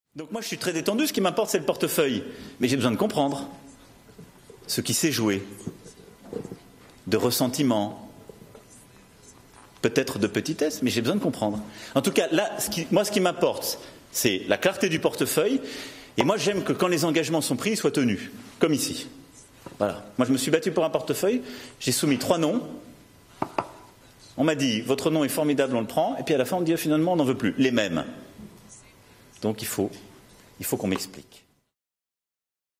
Rejet de la candidature de Goulard: Macron a "besoin de comprendre" | AFP Extrait
Emmanuel Macron réagit lors d'une conférence de presse à Lyon au rejet de la candidature de Sylvie Goulard à la Commission européenne. Le président français dit avoir "besoin de comprendre" les raisons, évoquant d'un ton acide "ce qui s'est joué de ressentiment, peut-être de petitesse" dans le vote des eurodéputés.